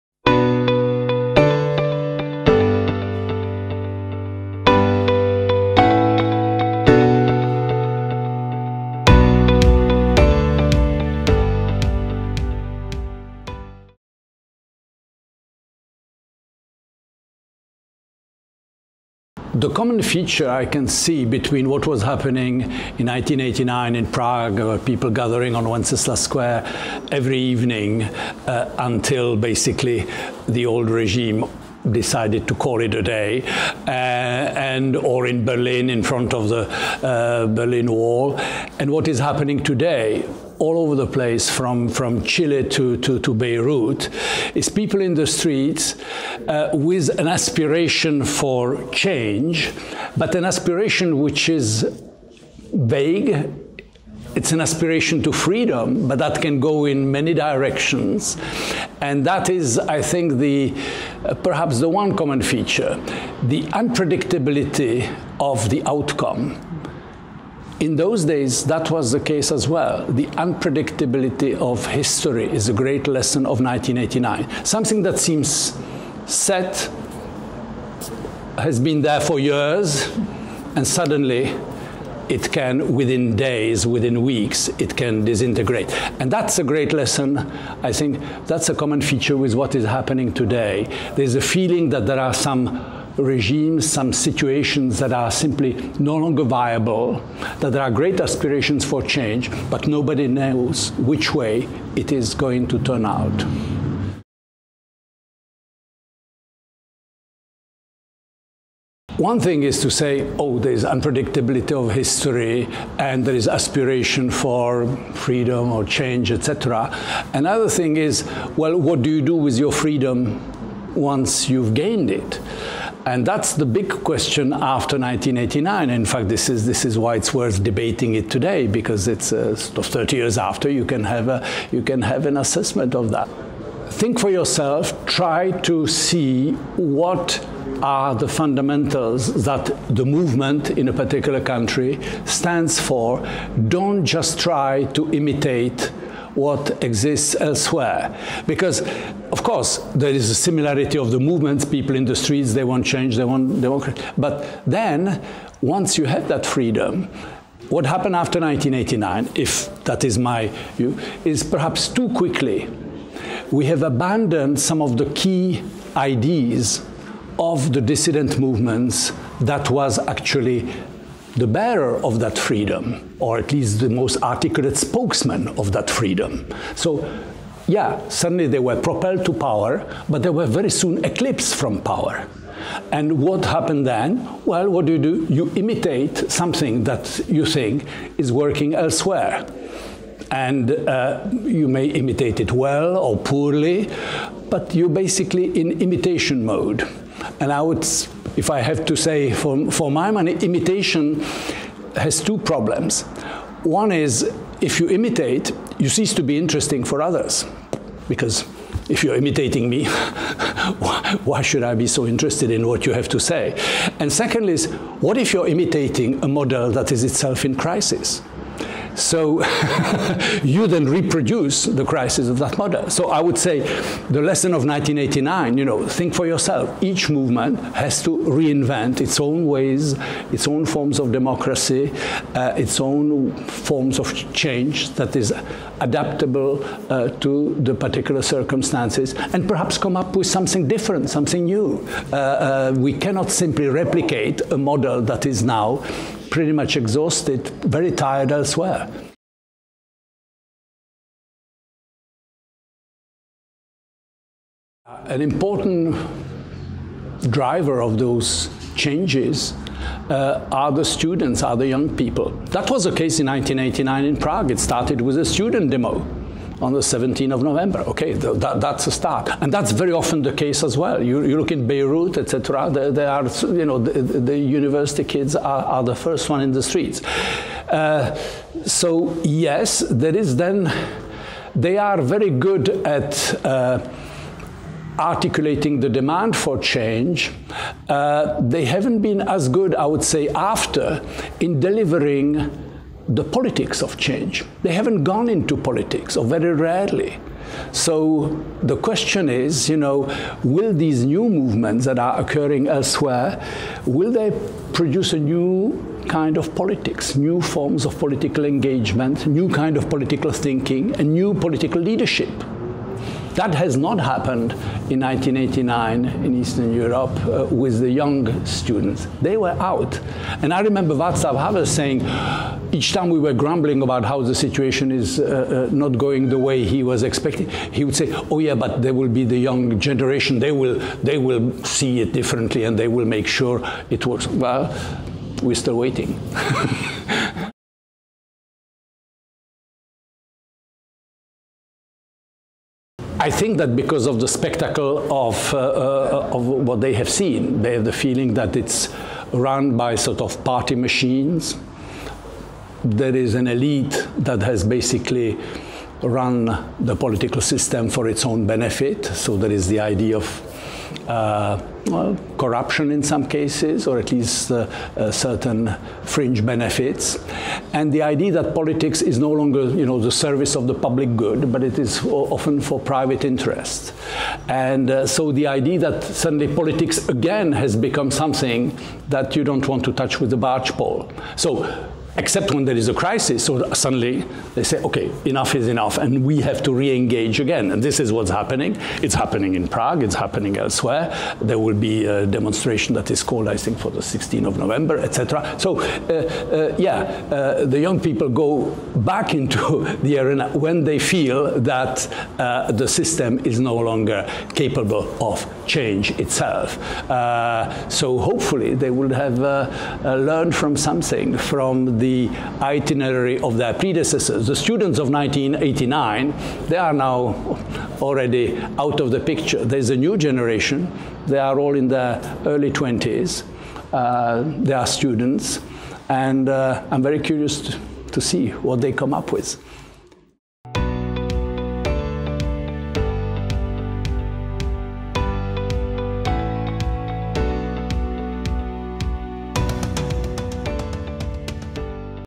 Jacques Rupnik: 1989 and Today
Jacques Rupnik is a Prague-born French historian and a political scientist focusing on the history and politics of Eastern and Central Europe. He was educated, among others, at the University of Paris and at Harvard, is currently Research Professor at CERI-Sciences Po in Paris as well as visiting professor at the College of Europe in Bruges. He was advisor to president Vaclav Havel in the 1990’s, Executive director of the International Commission for the Balkans, Carnegie Endowment for International Peace (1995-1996) and drafter of its report Unfinished Peace (1996), member of the Independent International Commission on Kosovo (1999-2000) and co-drafter of The Kosovo Report (Oxford UP, 2000). Among his more recent positions held, he was advisor to the European Commission (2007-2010), member of the board of the Institute for Historical Justice and Reconciliation in The Hague (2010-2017) and member of the board of directors of the European Partnership for Democracy in Brussels (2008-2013).